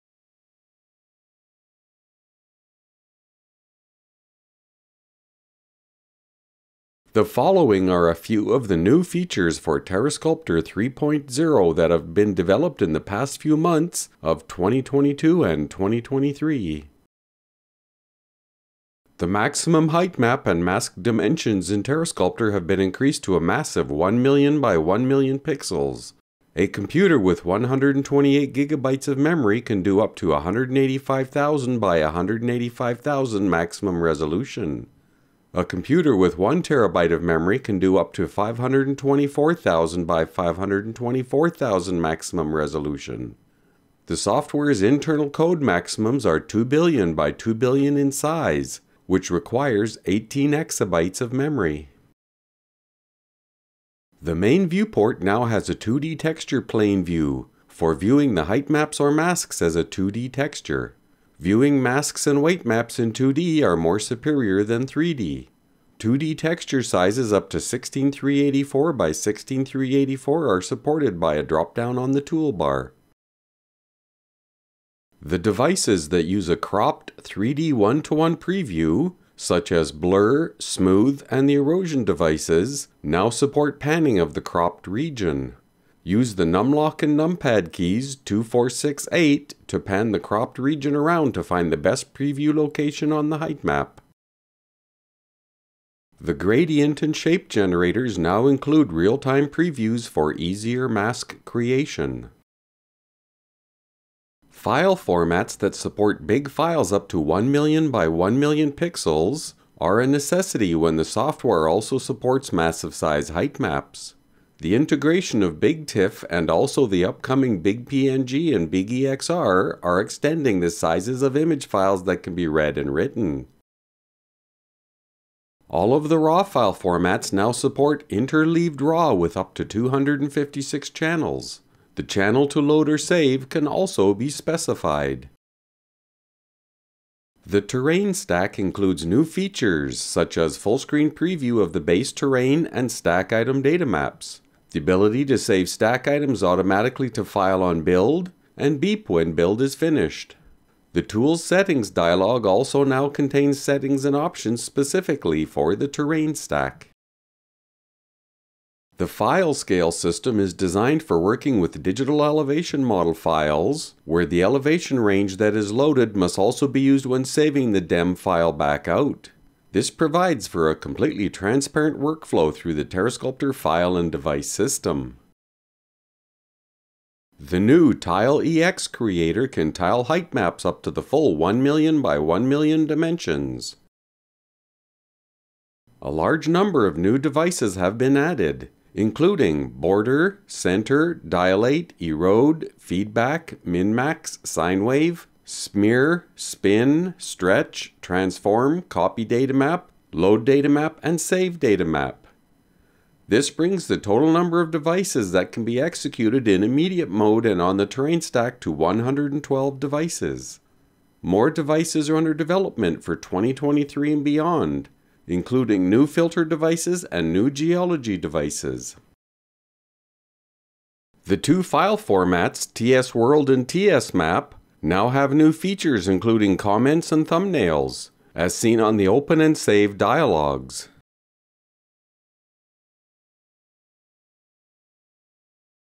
TS New Features for 2023
TerreSculptor New Features for November 2022 through Jan 2023.

Videos and Images:

Numerous Pixabay Images Used.